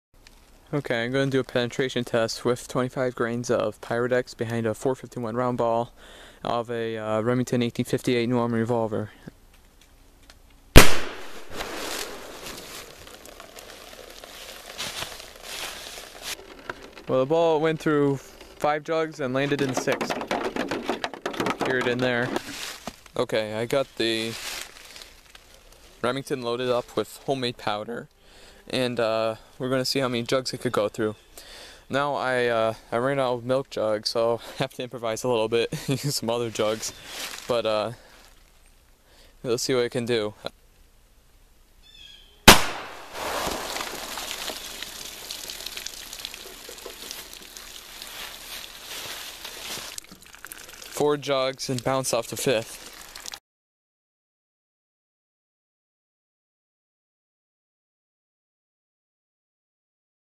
Homemade Black Powder vs Pyrodex
Here is a penetration comparison of homemade black powder and pyrodex black powder substitute. I Hope you enjoy the video